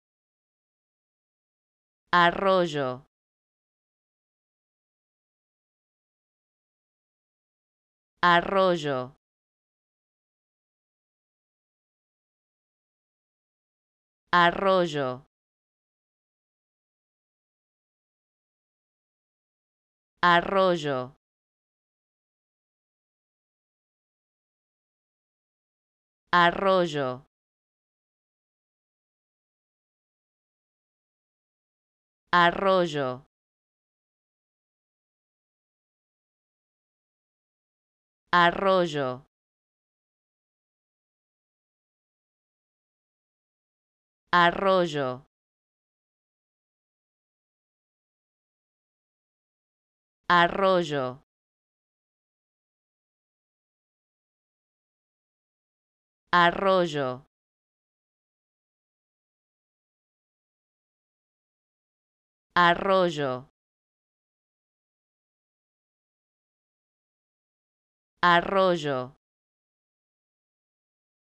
Como Pronunciar ''Arroyo'' (Stream) Correctamente en Español
Escucha la pronunciación de ''Arroyo'' y aprende a decirlo correctamente en Español.